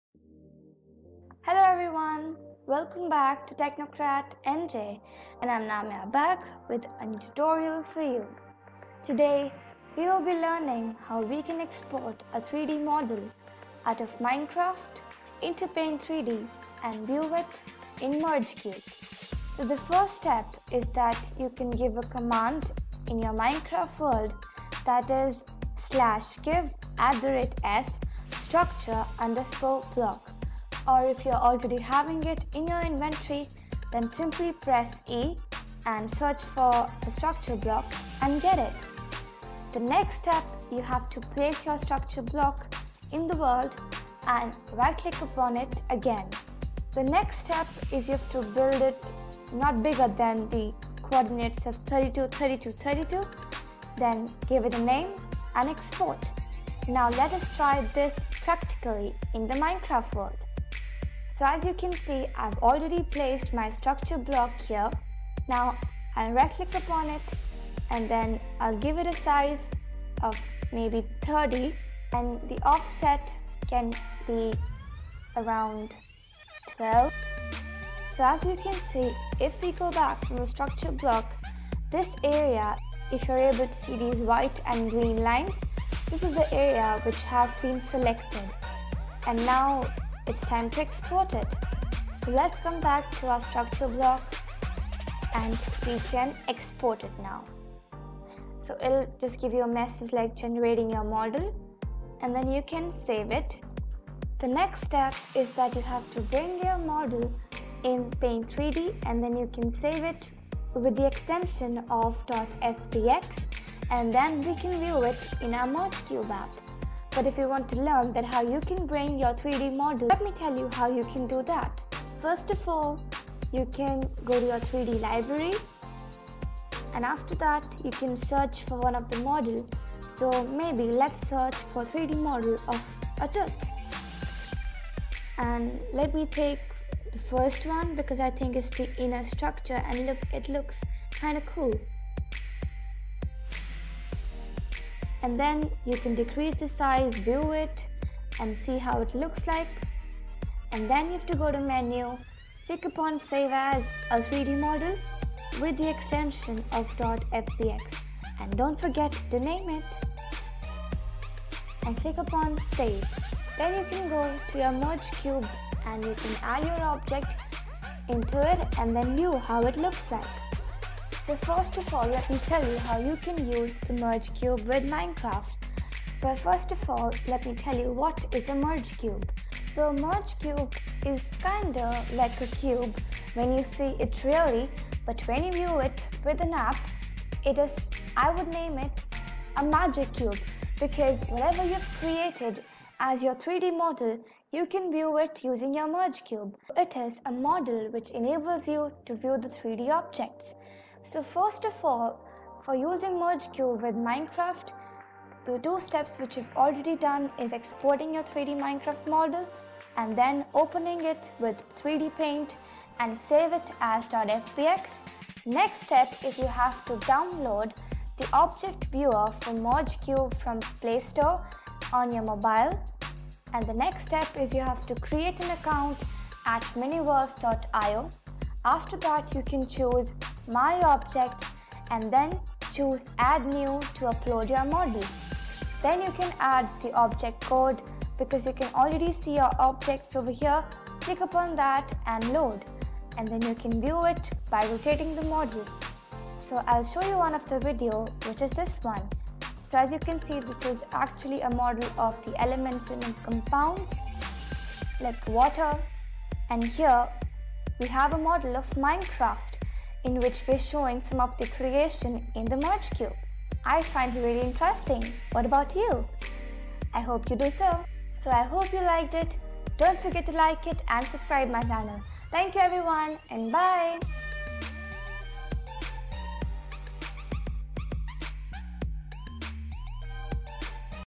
AR/VR With Minecraft And Merge Cube
In this video we will learn how we can export a 3D Model from Minecraft into Paint 3D and view it in Merge Cube. Happy Learning!!!